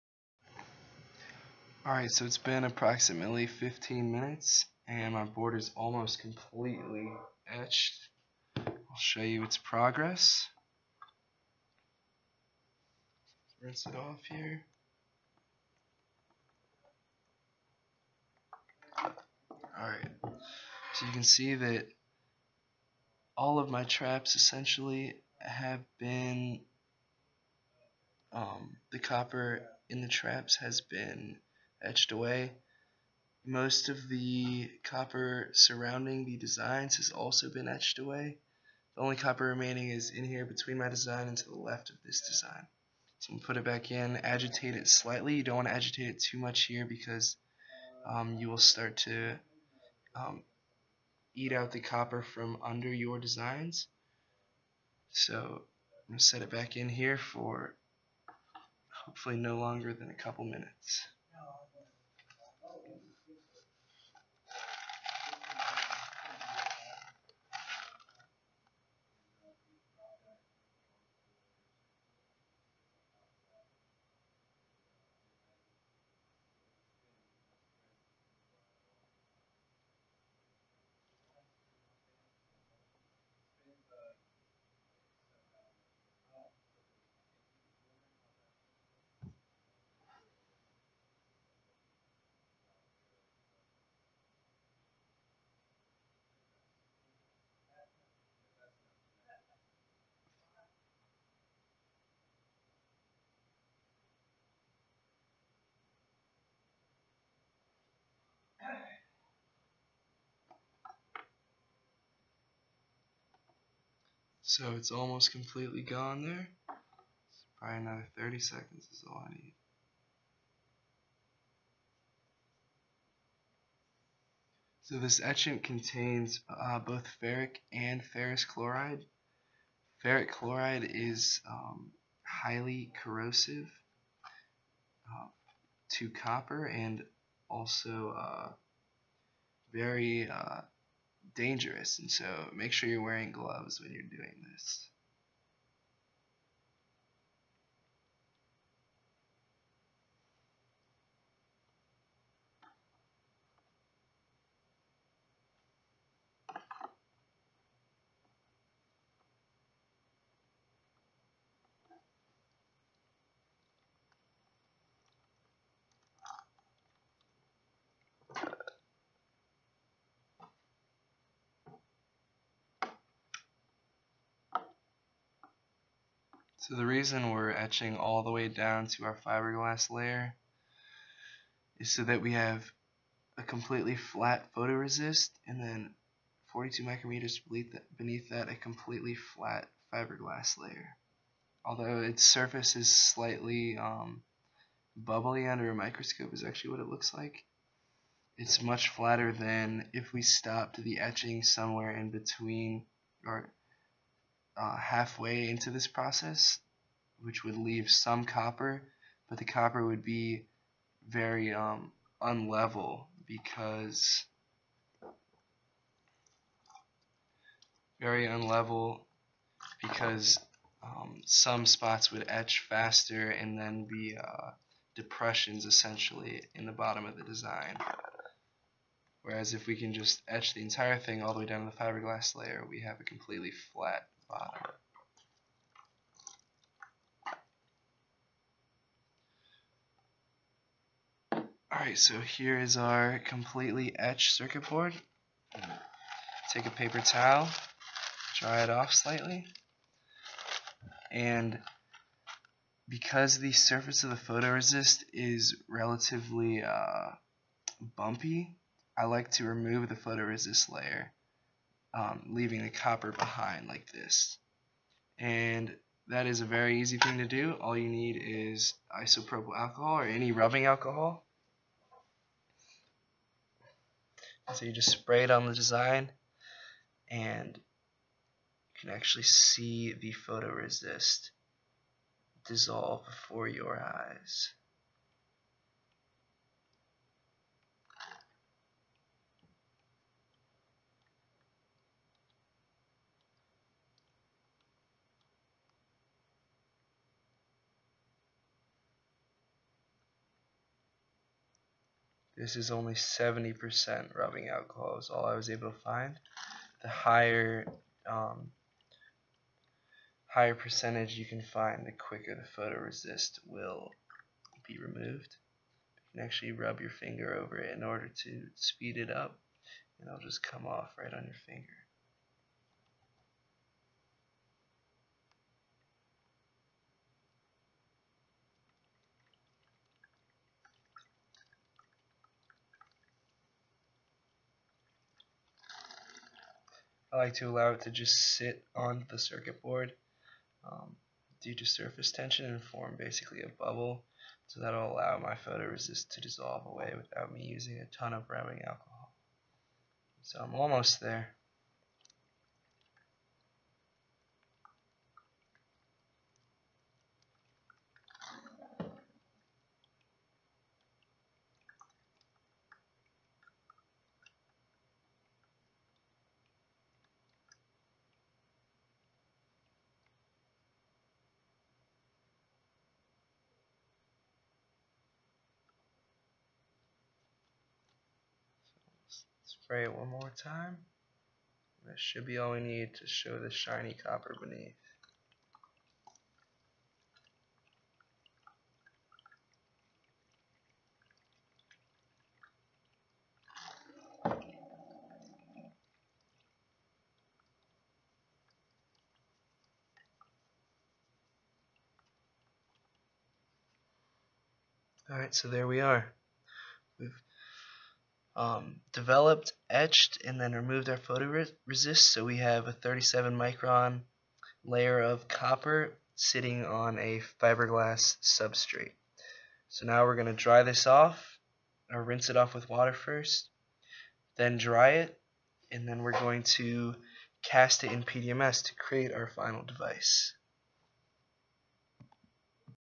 Step 6b: Etch Your PCB
Complete the etching of your PCB. Then remove any remaining photoresist from your design.